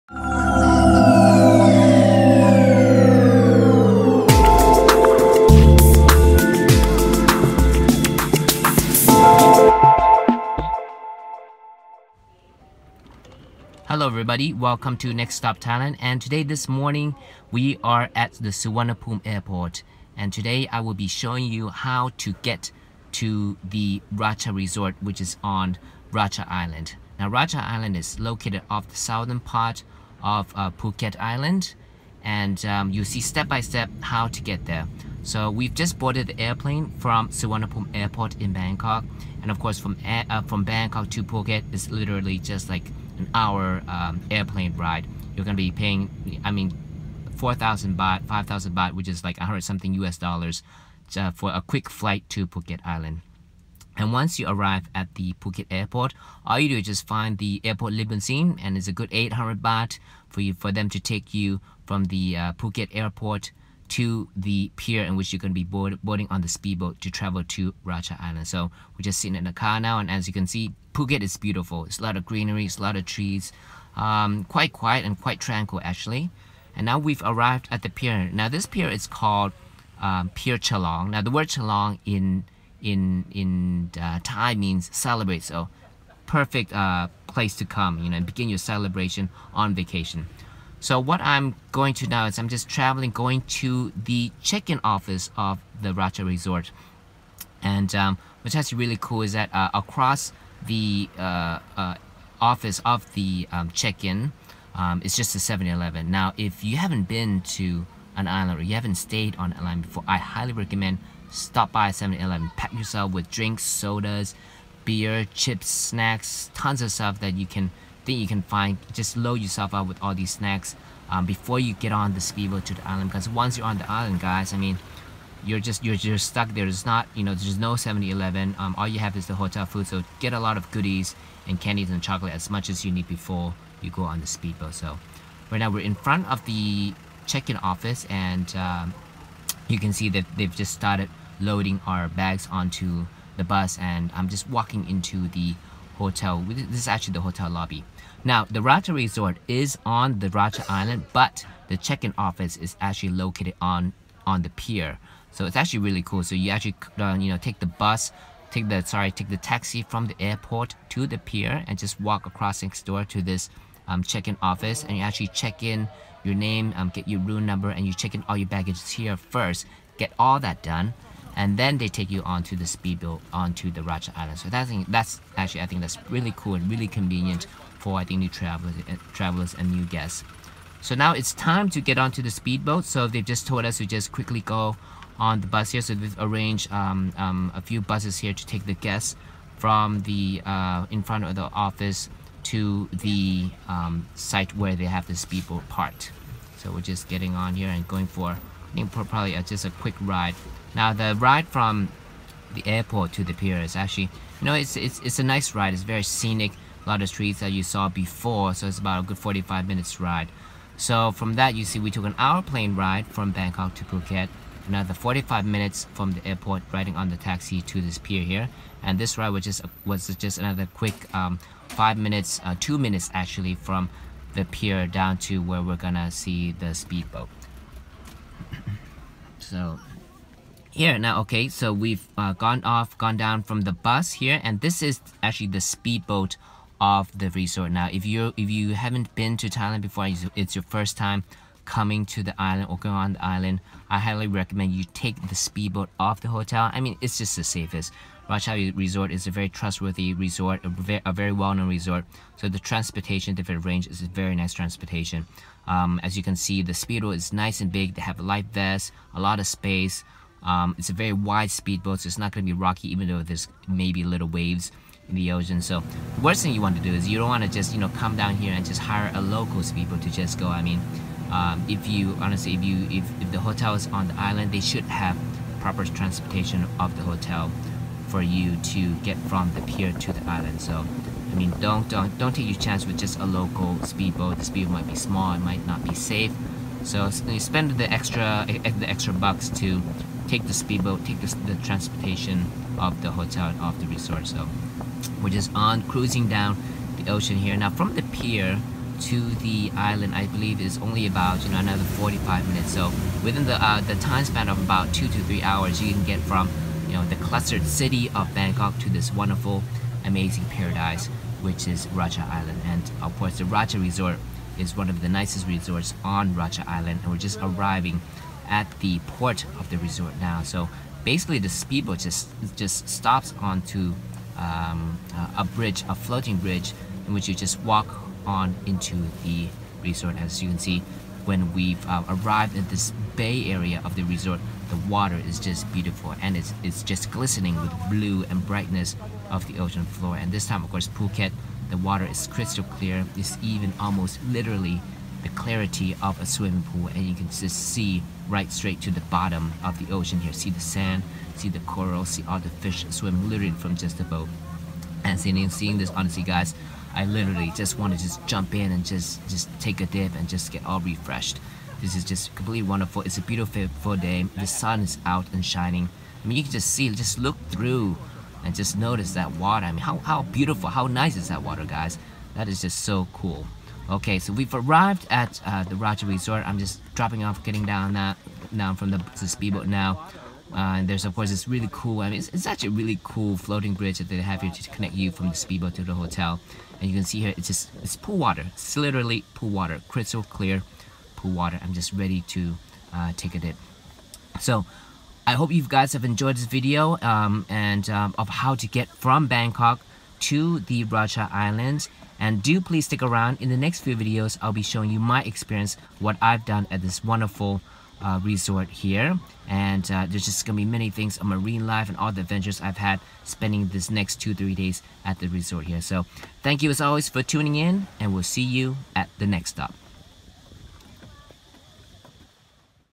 The Racha Resort Part 1 - Racha Island - Phuket - Thailand
For this first part I will show you how to get from Bangkok to The Racha Resort on Racha Island in Phuket, Thailand.